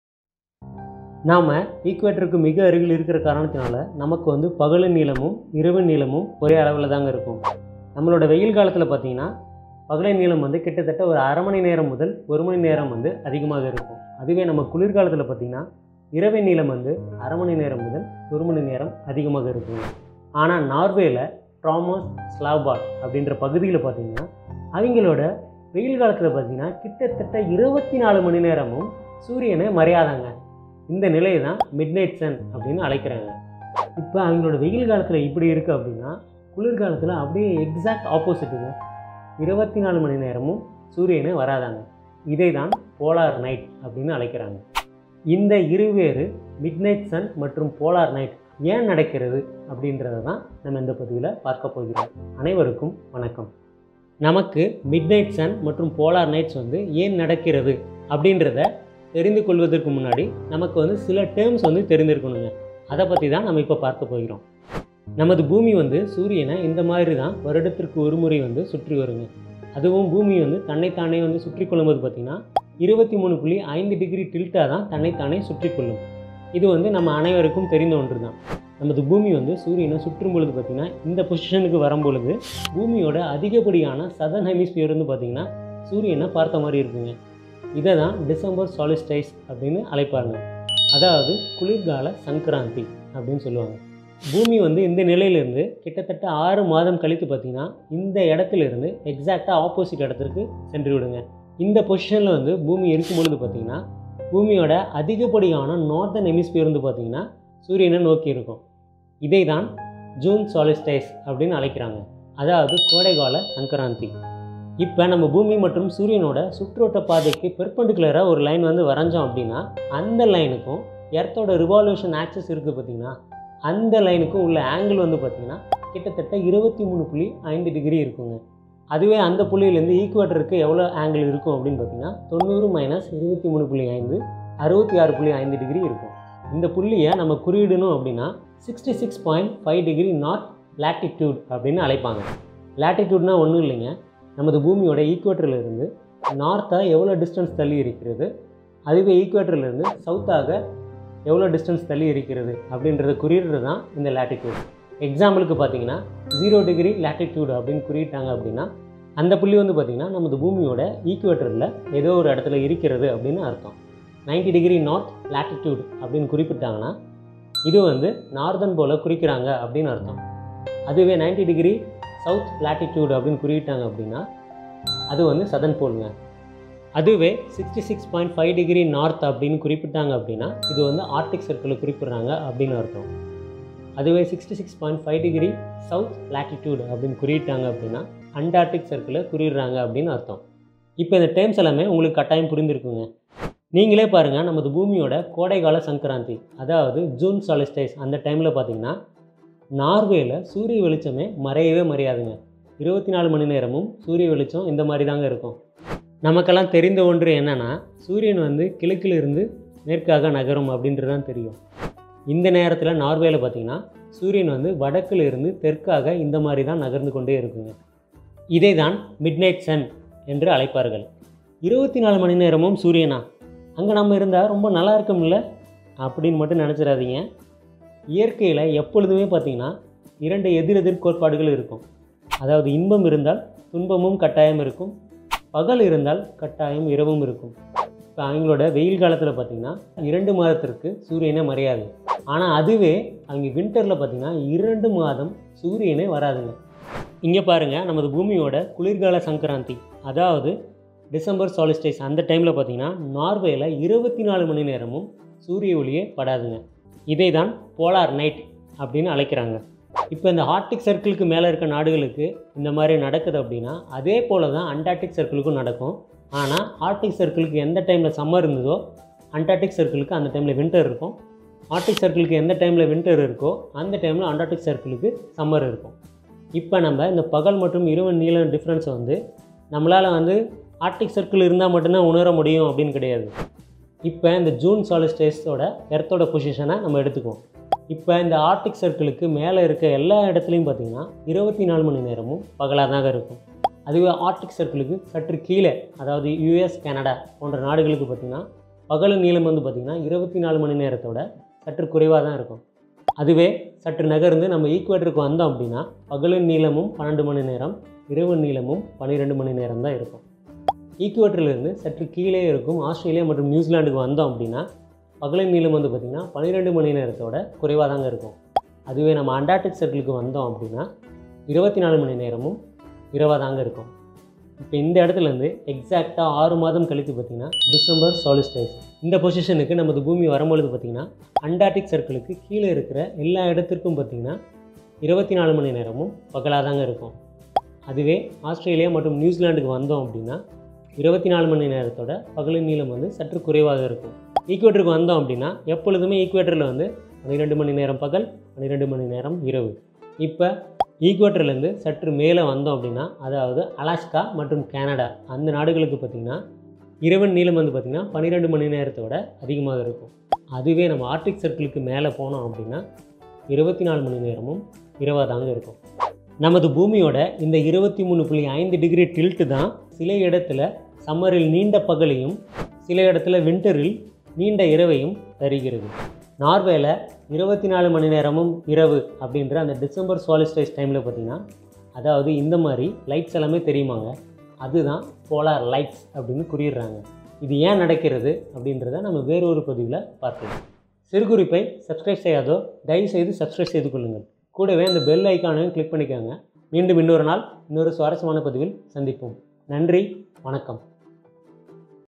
Mid Night sun 24 மணி நேரமும் பகல் | Polar Night 24 மணி நேரமும் இரவு | Norway Explained | Tamil
This Video Explains about 24 hours of Sunlight during Summer and 24 hours of darkness during winter on Norway. How this strange thing only happened to Norway all explained in Tamil